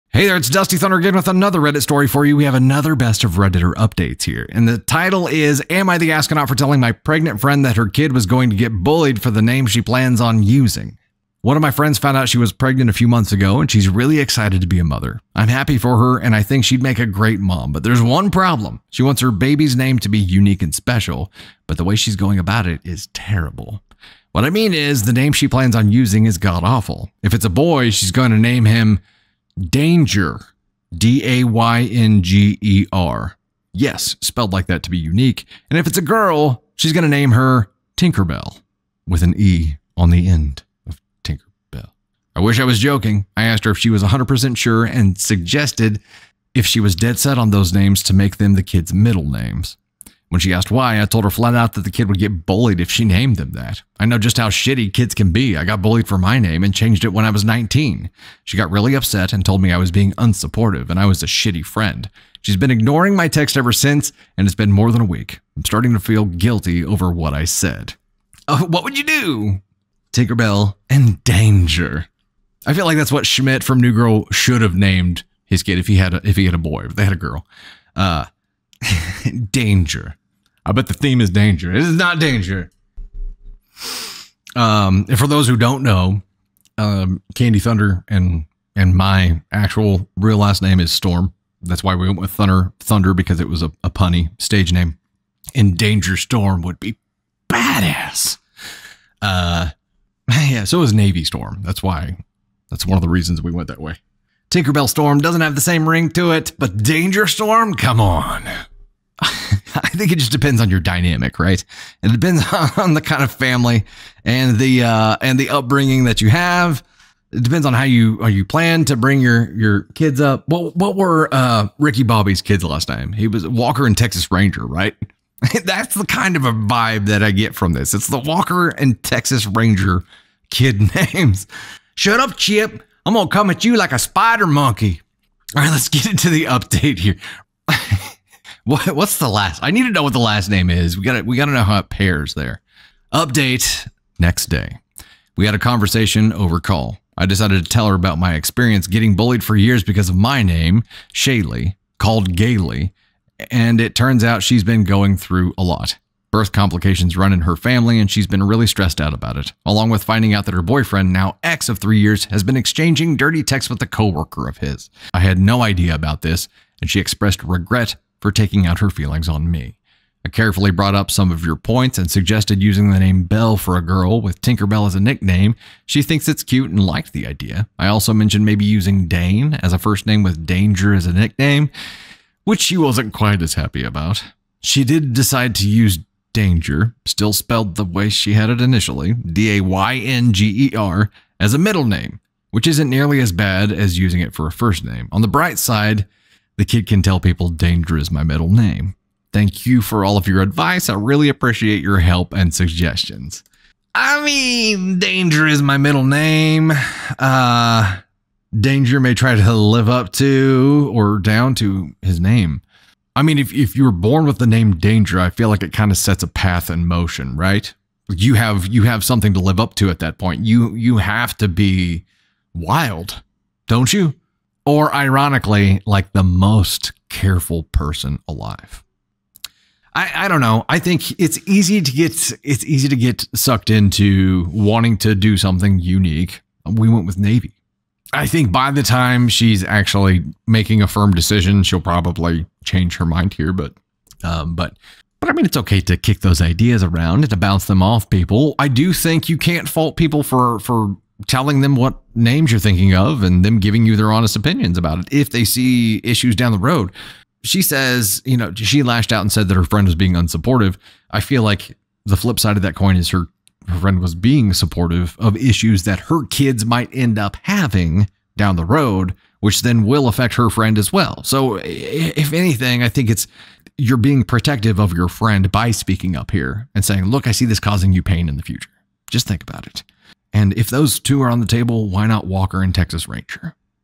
AITA for telling my pregnant friend that her kid was going to get bullied for their name?
AITA for telling my pregnant friend that her kid was going to get bullied for the name she plans on using?